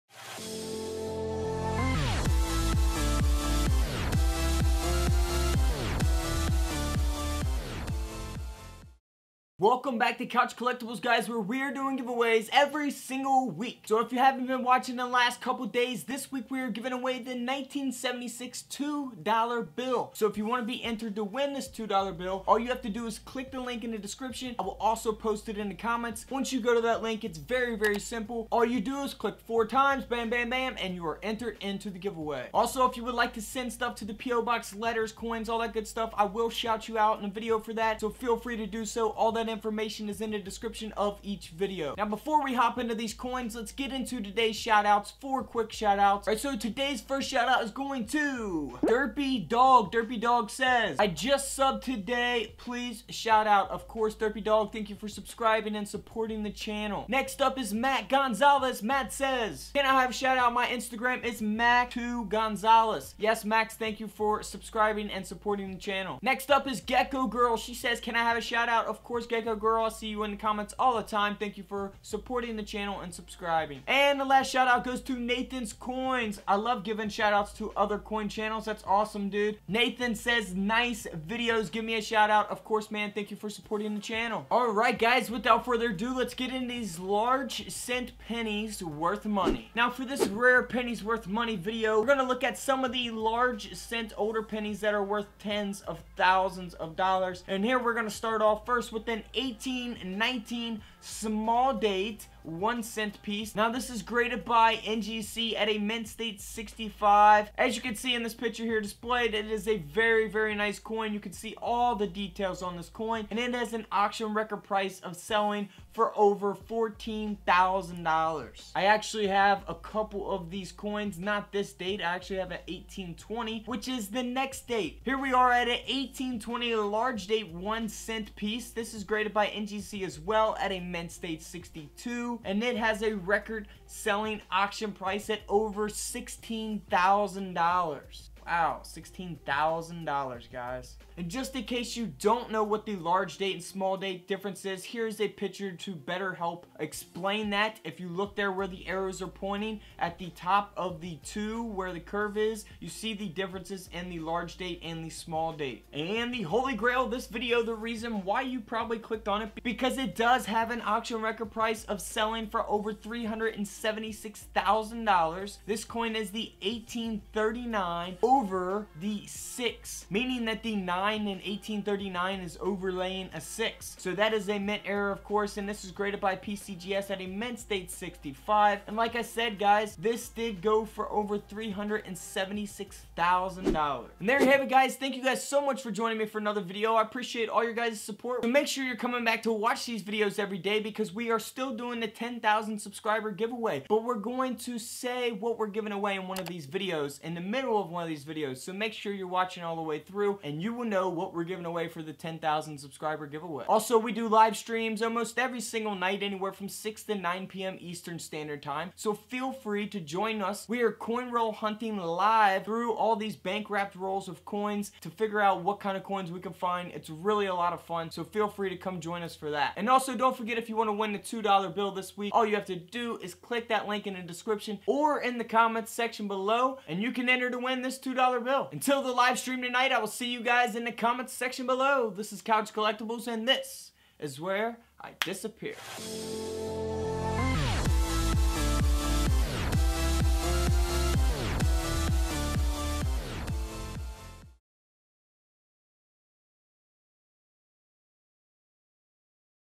$376,000 LARGE CENT PENNIES WORTH MONEY - MATRON HEAD LARGE CENT PENNY!!!
Searching for rare coins continues to be a growing hobby. These are rare valuable large cent pennies worth money. We look at a $376,000 Matron Head Large Cent Penny that sold at an auction record! For more valuable old coins worth money tips give the video a thumbs up! Keep coin roll hunting and always remember you can find rare and silver coins without going to a coin dealer or coin shop and spending money!